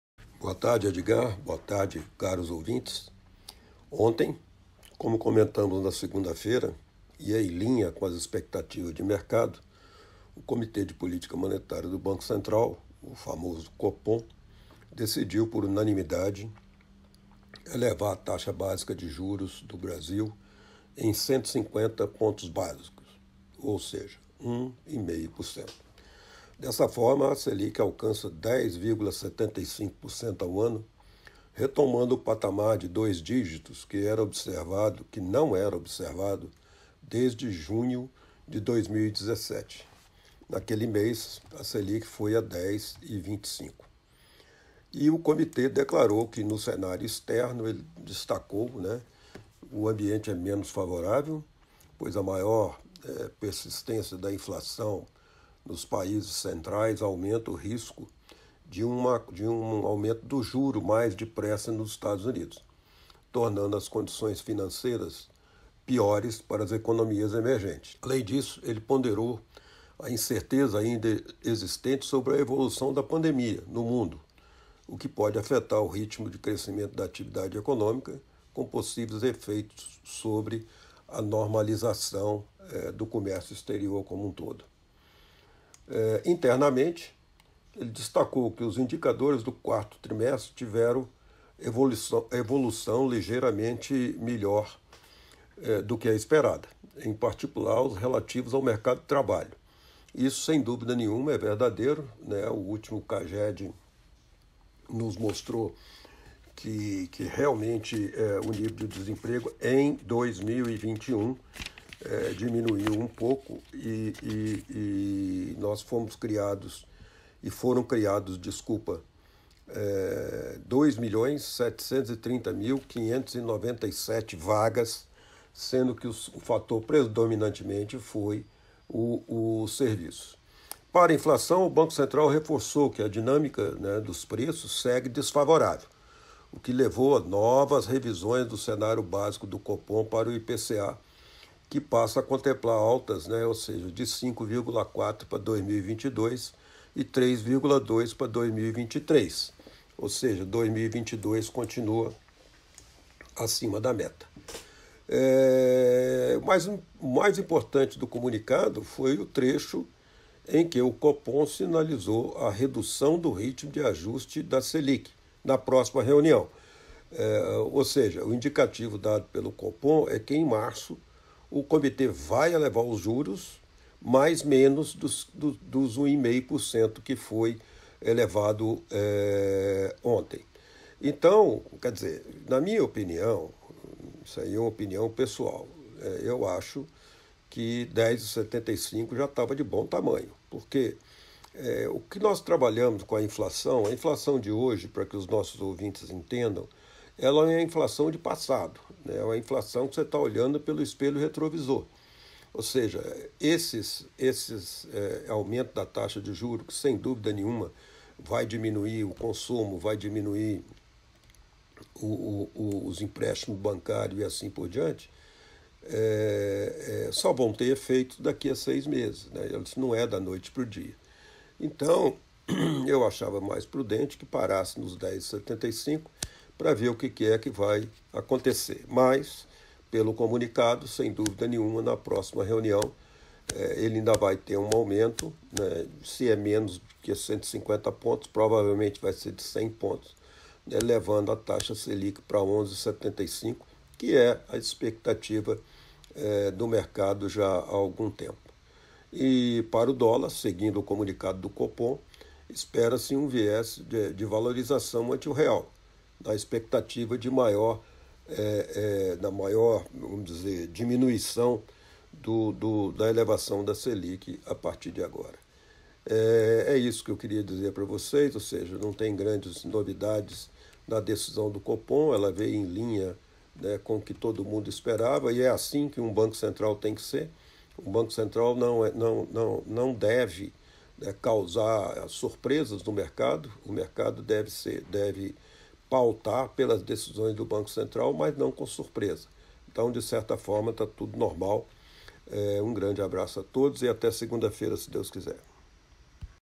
GUSTAVO DO VALE COMENTA A ELEVAÇÃO DA TAXA DE JUROS PARA 10,75
| ECONOMIA |